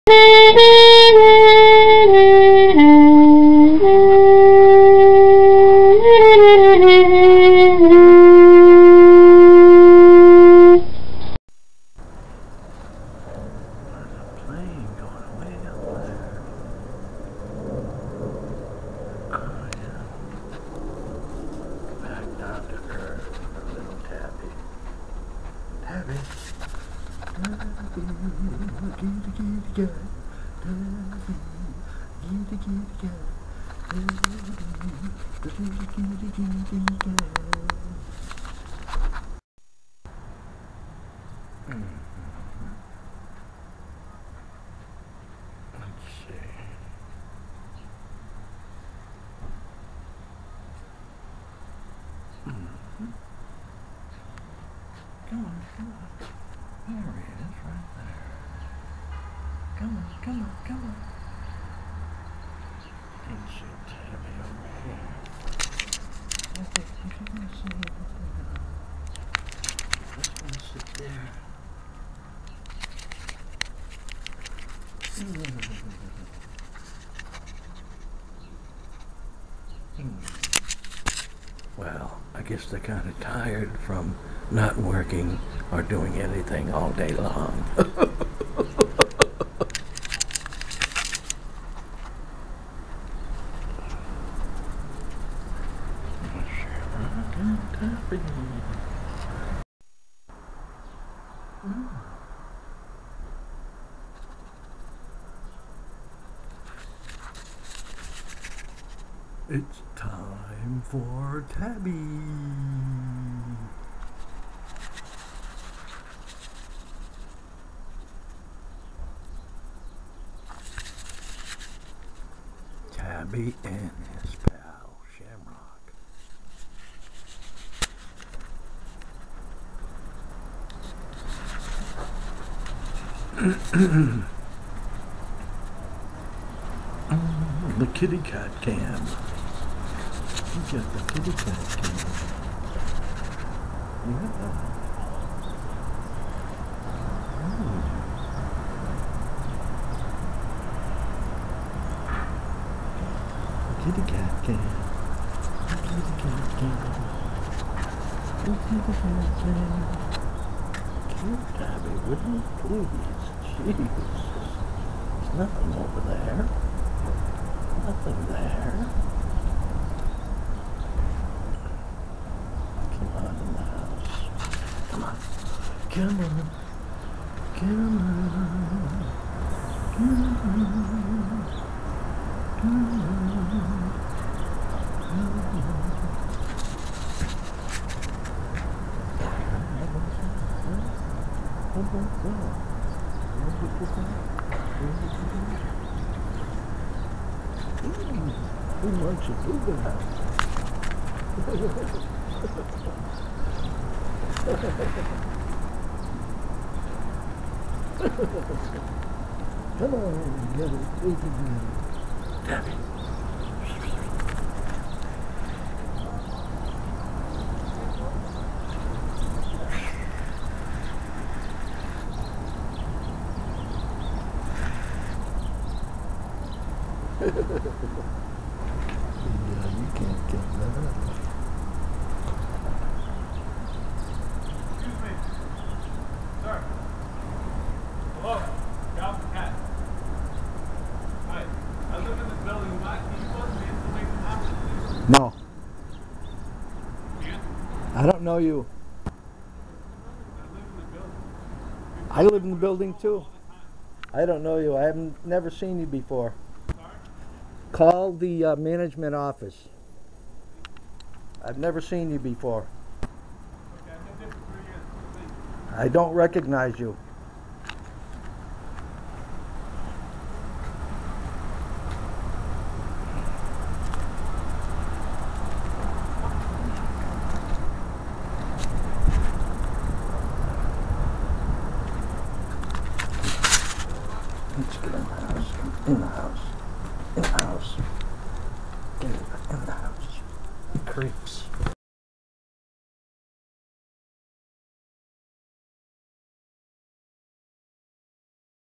People Beware
This mini drama begins like movies of this type do,very uneventful and casual, until near the end when a rude stranger attempts to get me to open the main door to allow him entrance. He was someone I have never seen and I do not let strangers in this building. He said he lived here but never mentioned his name, a very suspicious action. You do not see him but the audio is very clear in the tense dialogue trying to get into buildings around here and use scams such as this to get in the building.So thats how it is in Boston these days,just a few days ago two women were stabbed in the Back Bay, a chic shopping area of Boston,right near the Marathon bombing of April 15 2013 as a matter of fact.